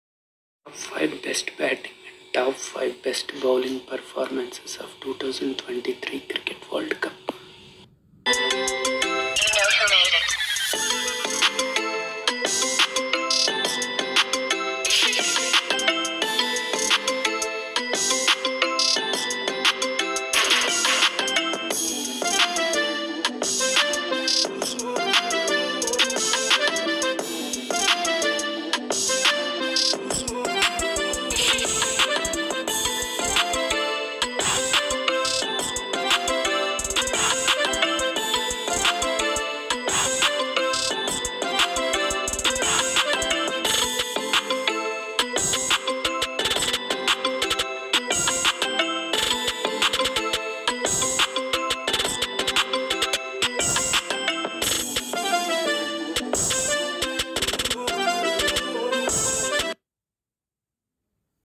Unveiling the Phenomenal 5: Best Batsmen and Bowlers of Cricket World Cup 2023!
In this video, we will be discussing the top 5 batsmen and top 5 bowlers who have made a significant impact in the 2023 World Cup.

Cricket fans around the world have witnessed some extraordinary performances from both batsmen and bowlers in this highly anticipated tournament. In this video, we will be highlighting the cricketers who have stood out and made a lasting impression on the cricketing world.

Starting with the batsmen, we will delve into the top 5 players who have showcased their exceptional batting skills throughout the tournament. From explosive hitters to technically sound players, this list will cover a diverse range of talents who have consistently delivered for their respective teams.

Moving on to the bowlers, we will explore the top 5 performers who have left a lasting impact with their lethal bowling spells. Whether it's pace, spin, or accuracy, these bowlers have consistently troubled batsmen and played a crucial role in their team's success.

Throughout the video, we will provide in-depth analysis, statistics, and highlights of each players' performances, giving you a comprehensive

overview of their contributions to the 2023 World Cup.

So, if you're a cricket enthusiast or simply curious about the standout performers in the 2023 World Cup, this video is a must-watch! Join us as we celebrate the incredible talent and skill displayed by the top 5 batsmen and top 5 bowlers in this thrilling tournament. Let's dive into the world of cricket and explore the best of the best in the 2023 World Cup!

TOP 5 BEST BATTING performance
TOP 5 BEST BOWLING Performance
2023mens cricker worldcup